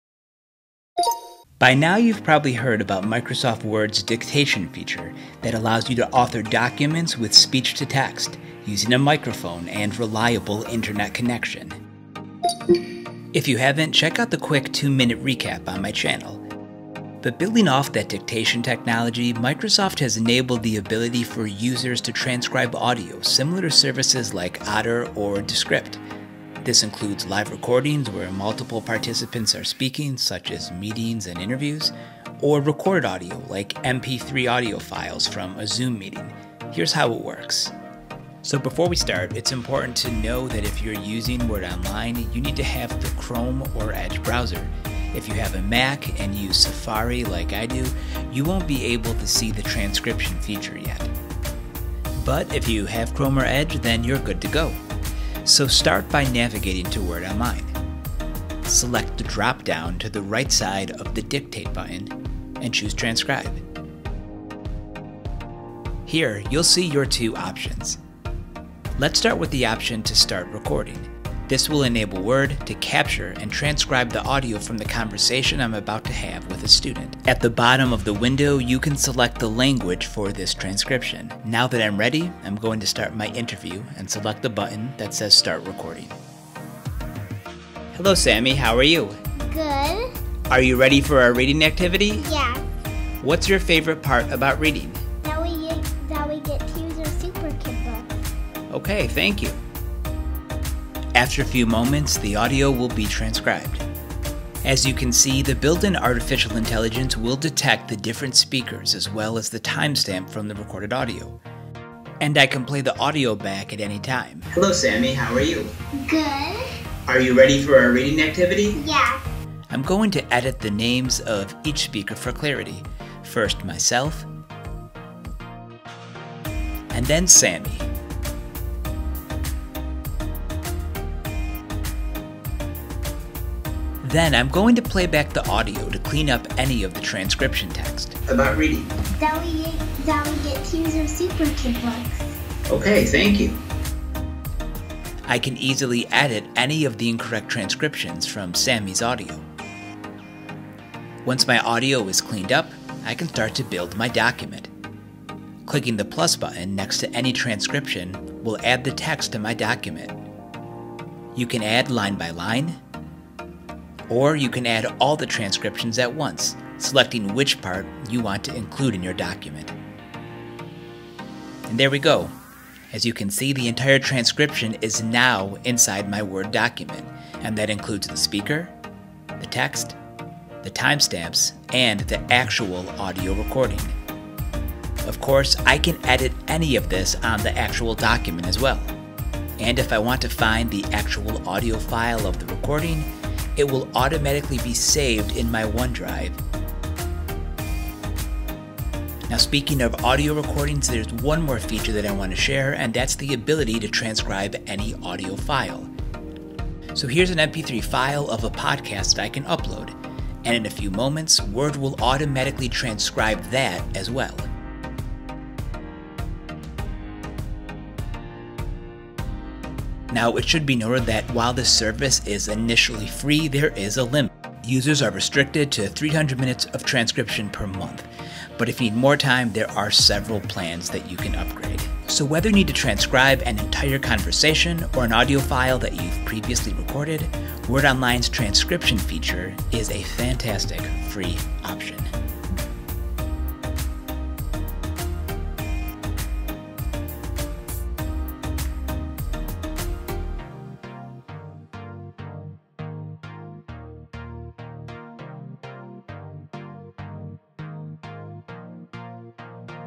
Microsoft Word: Transcribe Audio For FREE!
Are you an Office 365 subscriber that's looking for an easy way to transcribe recordings?  Check out the Transcribe dictation feature in Microsoft Word Online which provides a similar service to Otter or Descript.

The transcribe feature converts speech to a text transcript with each speaker individually separated. After your conversation, interview, or meeting, you can revisit parts of the recording by playing back the timestamped audio and edit the transcription to make corrections. You can save the full transcript as a Word document or insert snippets of it into existing documents.

You can transcribe speech in two ways:
* Record directly in Word
* Upload an audio file

Chapters:
0:00 Overview
1:00 Transcribe a Recorded Conversation
1:51 Editing the Transcript
3:11 Locating the audio file of the recording
3:28 Transcribing an MP3 audio file (Podcast)